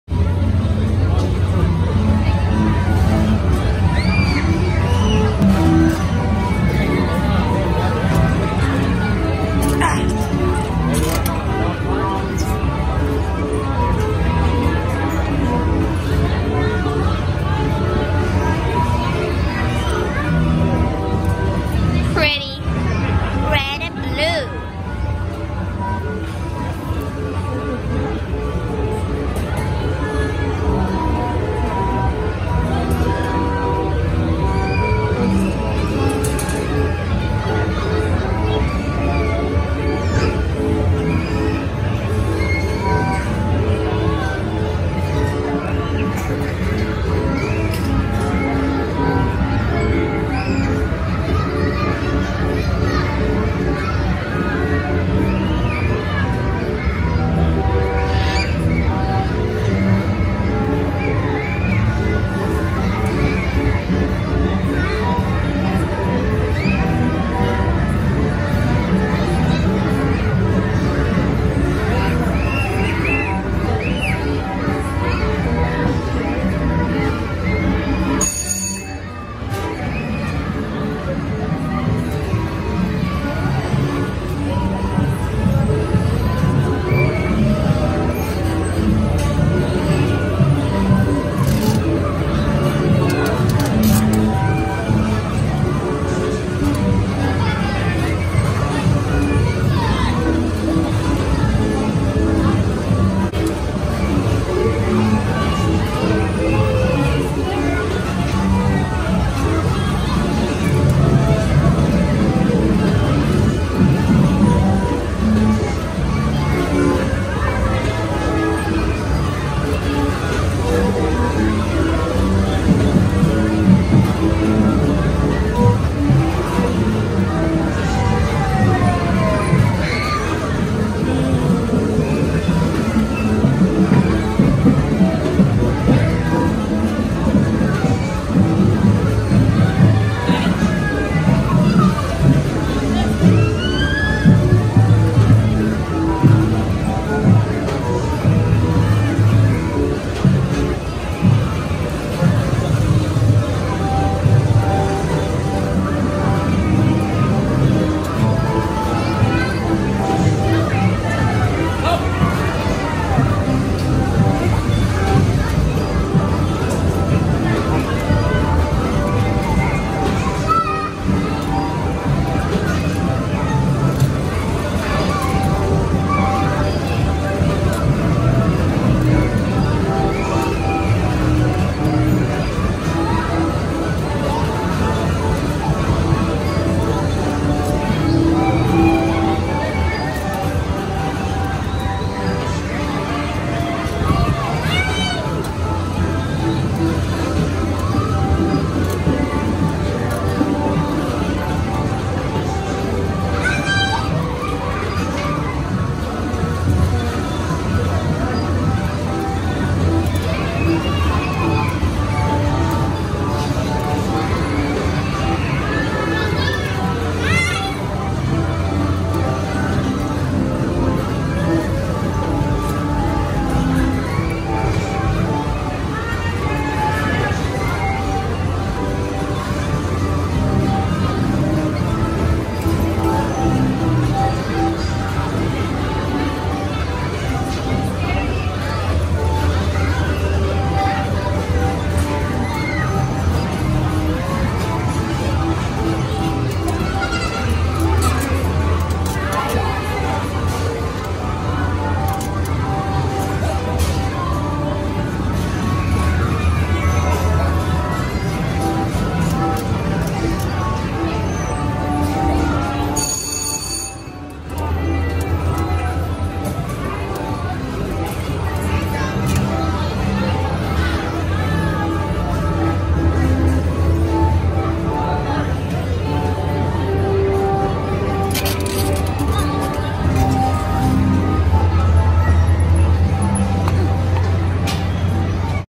Carousel inside horse
Black hair
Blue and red
Gold ears
Blue and red green stripes dress
￼black eyes